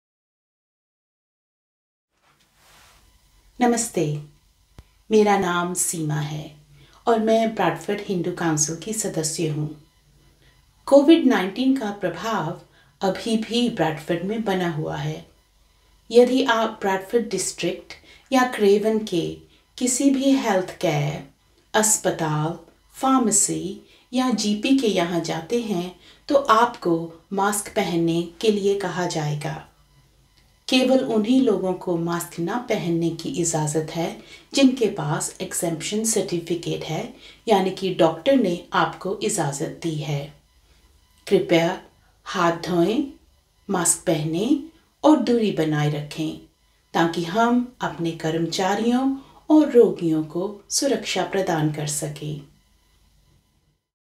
Hospitals and Health Centres (Hindi)
This video was recorded by Seema Buttoo from Bradford Hindu Council in Hindi.

COVID-19 is still circulating in Bradford. When accessing health services, hospitals, pharmacies and GP practices across Bradford District and Craven, you will be asked to wear a face covering unless exempt.

To keep our staff and patients safe, please remember to wash your hands, cover your face and keep a safe distance.